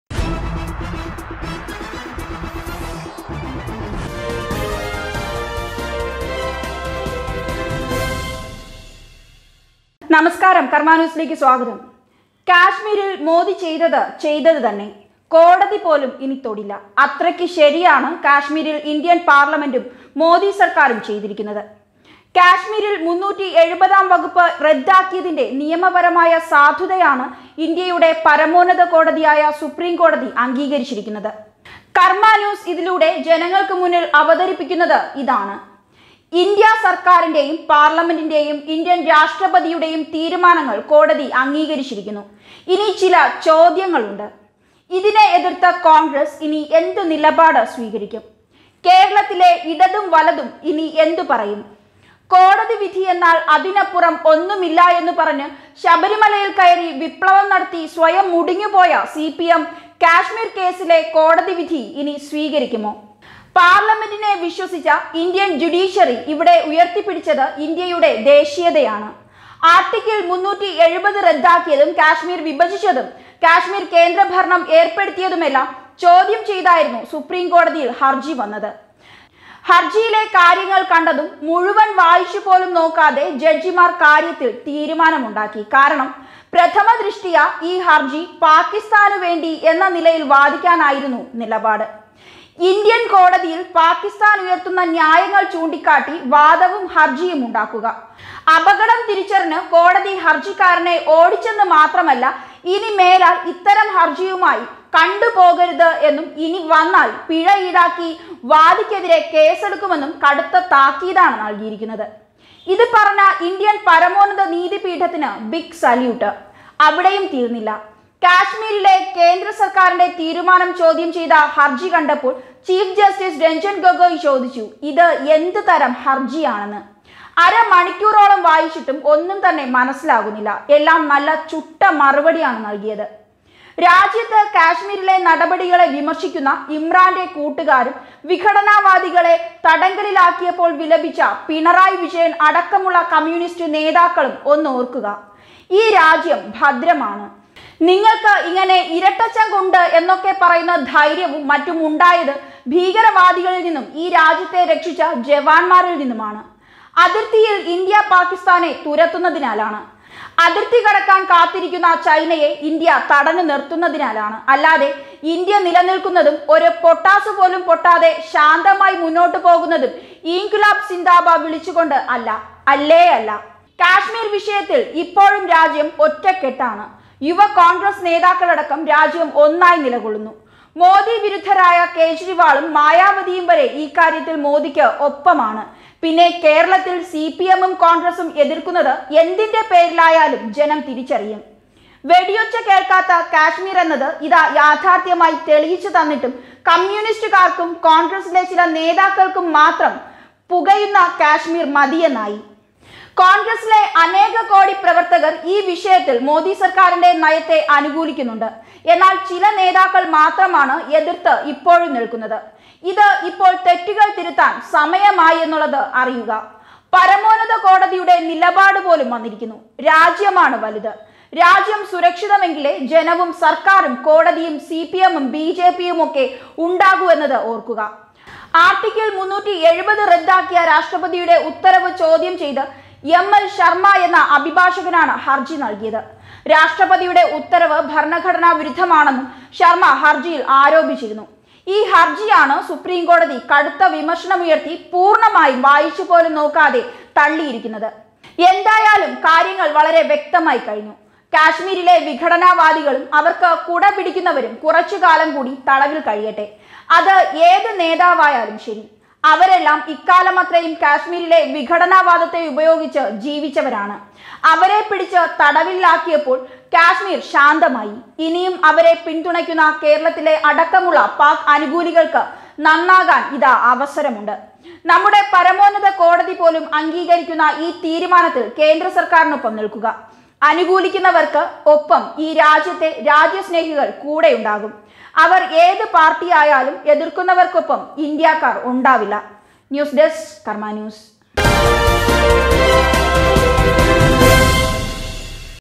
കാശ്മീരിൽ ചെയ്തത് ചെയ്തത് തന്നെ. കോടതി പോലും തൊടില്ല| karmanews
കാശ്മീർ: മോദിക്കൊപ്പം സുപ്രീം കോടതിയും, രാജ്യം വേണ്ടാത്ത കമ്യൂണിസ്റ്റ്, കോൺഗ്രസ് കടൽ കിഴവന്മാർക്ക് തിരിച്ചടി.